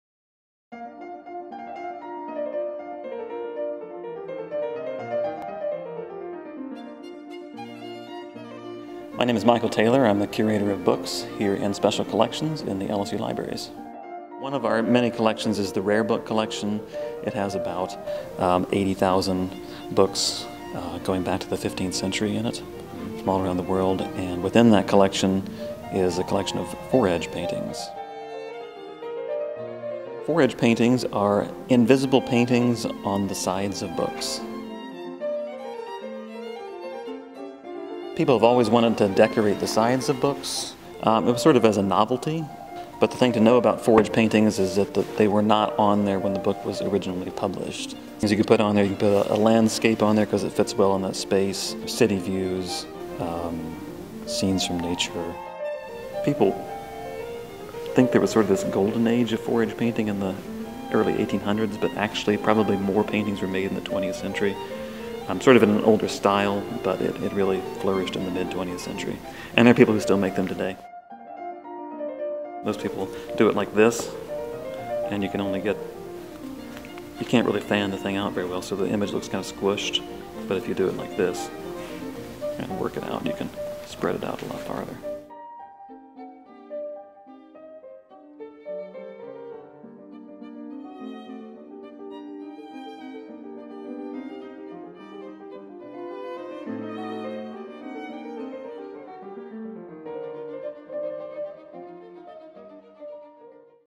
Painting On The Edge - LSU Libraries Special Collections
We hear a lot about “vanishing” Louisiana these days. Our coast, towns, and wildlife are at risk from hurricanes, oil spills, rising sea levels, and changes in land management, and elements of our state’s distinctive culture and history are gradually disappearing. Four books in the LSU Libraries’ Special Collections reveal a little of what has been lost… but in a way that will surprise you!

The LSU Libraries Special Collections are open to the public from 9am-5pm Monday through Friday.